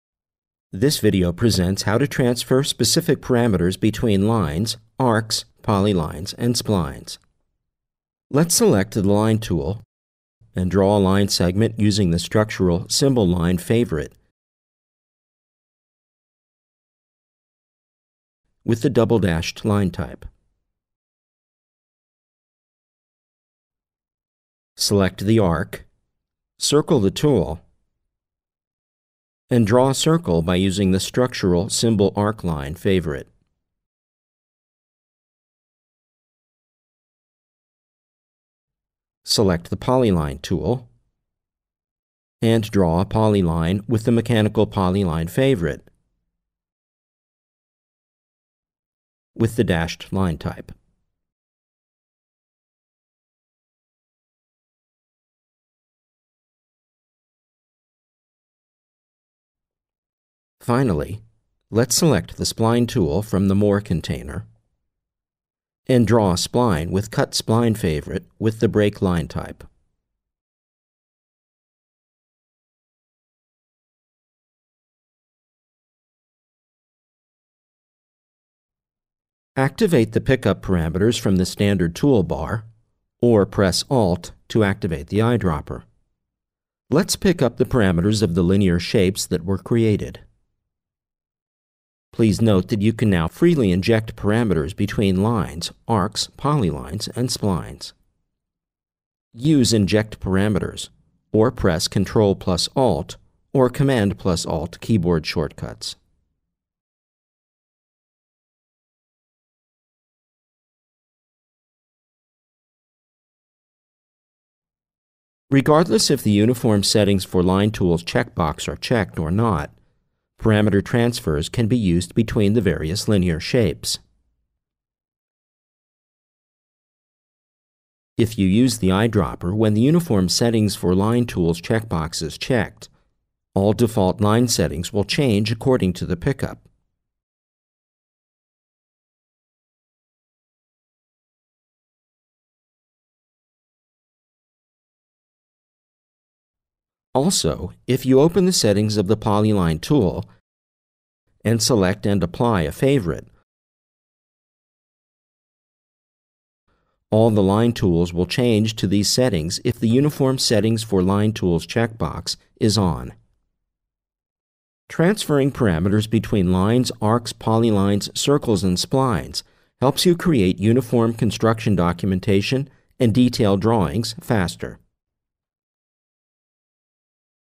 ARCHICAD 21 - Parameter Transfer Between Lines
This video presents how to transfer specific parameters between lines, Arcs, Polylines, Circles and Splines in ARCHICAD 21. Transferring parameters helps you create uniform construction documentation and detail drawings faster.